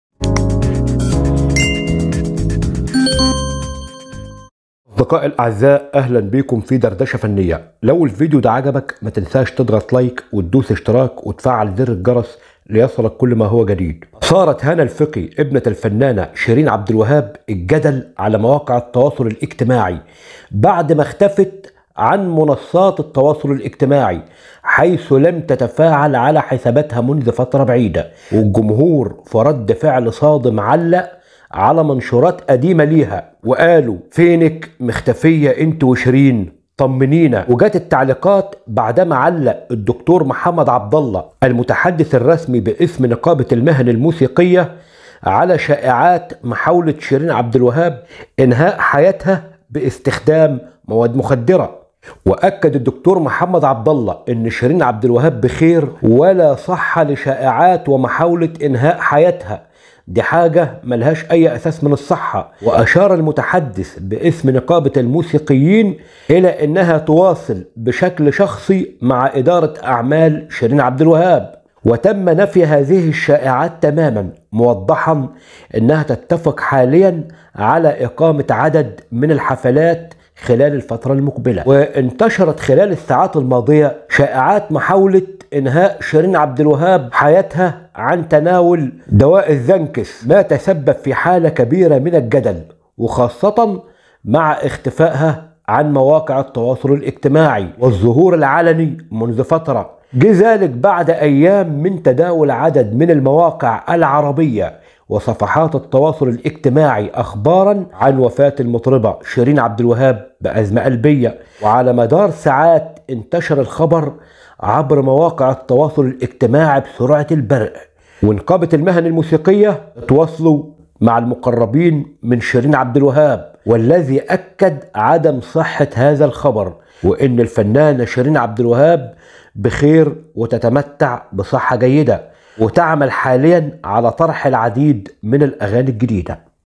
محاولة انتحار شرين عبدالوهاب.. وقصة اختفاء ابنتها في ظروف غامضة
الفنانة شرين شرين_عبد_الوهاب اطلقت عليها اشاعة انها حاول الانتحار ونفى هذا الكلام الدكتور#محمد_عبدالله المتحدث باسم نقابة_المهن_الموسيقية.. وقال شرين بخير والاشاعة ليس لها اساس من الصحة كما اطلق على ابنتهاانها اختفت